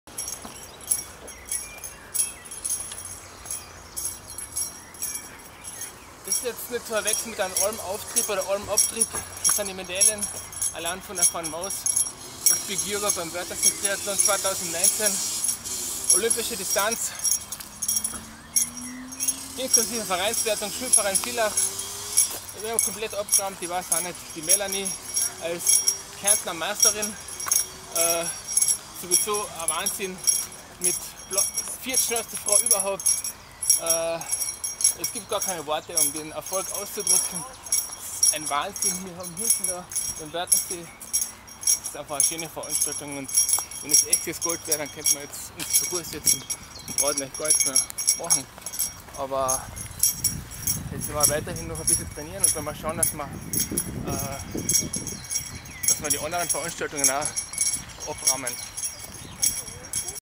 Bigjuergo Carinthian Agegroup Champion Postrace Interview Wörthersee Triathlon 2019
road to kona
from zero to kona
what else?!

there can be fuckin only one!

BIGKONASLOTATTACK in PROGRESS ...